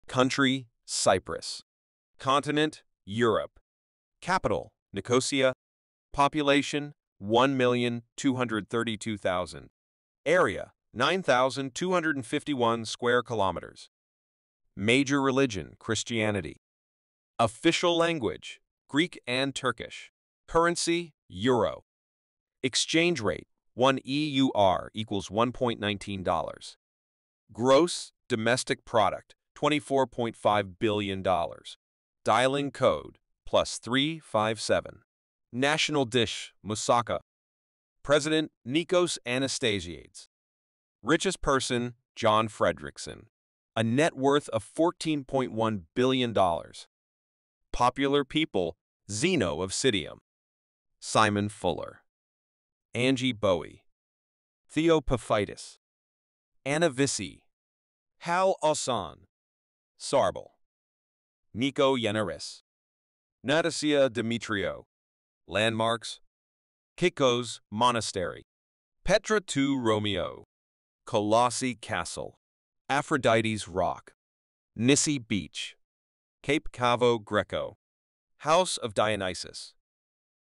Explore the Beauty and Culture of Cyprus | Landmarks, Cuisine, and More!
Step into the enchanting world of Cyprus, a captivating European island with a rich history and stunning landscapes. Join us on a virtual journey as we uncover the wonders of this Mediterranean gem. From the vibrant capital city of Nicosia to the breathtaking landmarks like Kykkos Monastery and Petra tou Romiou (Aphrodite's Rock), Cyprus offers a tapestry of cultural and natural treasures. Indulge in the mouthwatering national dish, Moussaka, and immerse yourself in the warm hospitality of its people. Discover the fascinating mix of Greek and Turkish influences, reflected in its official languages and diverse population. Get ready to be mesmerized by Nissi Beach's turquoise waters and explore the ancient history of Kolossi Castle and the House of Dionysus. Don't miss out on the chance to experience the magic of Cyprus from the comfort of your screen. Watch now and let Cyprus ignite your wanderlust!